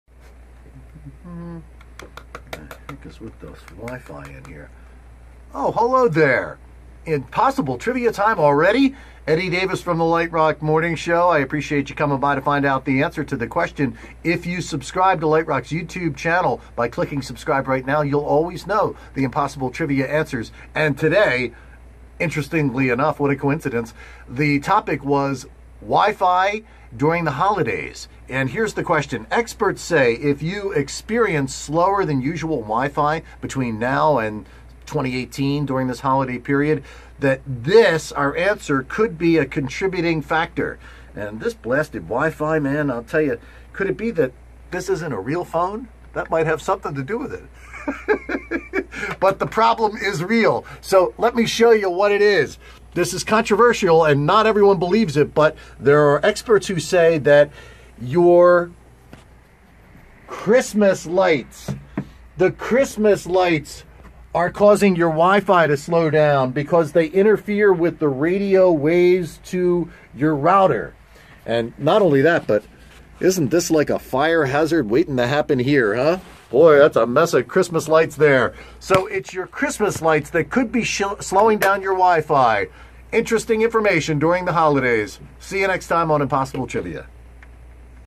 This Could Be Slowing Down Your Wi-Fi During the Holidays? Impossible Trivia
Today’s Impossible Trivia: Experts say if you experience slower wi-fi during the holidays, this could be a contributing factor.  What is it?